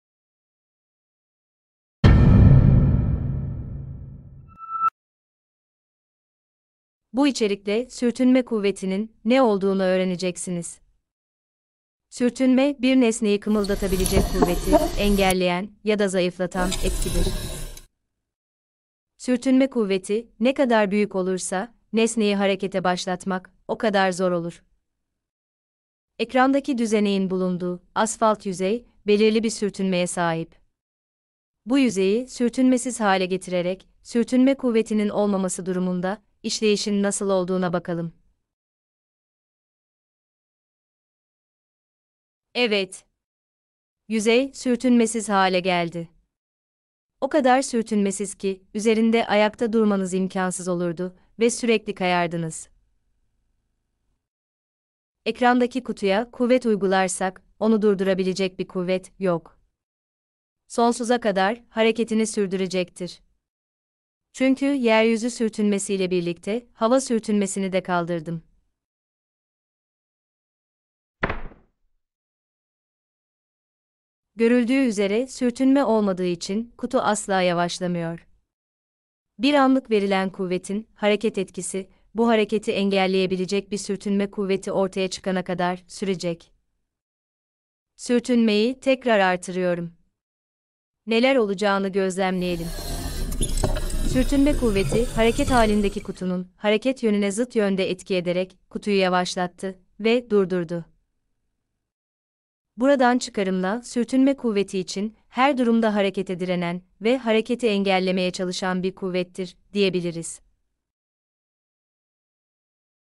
Sürtünme Kuvveti (Matematiksiz Anlatım) (4K UHD)
Sürtünme kuvvetini matematik bilgisi kullanmadan kolaylıkla öğrenmenizi sağlayacak animasyon için videoyu izleyebilirsiniz.

Sürtünme kuvveti nedir? Sürtünme kuvveti nelerden etkilenir? Sürtünme kuvveti hangi durumlarda ortaya çıkar? Sürtünme kuvvetinin nesneler üzerindeki etkisi nedir?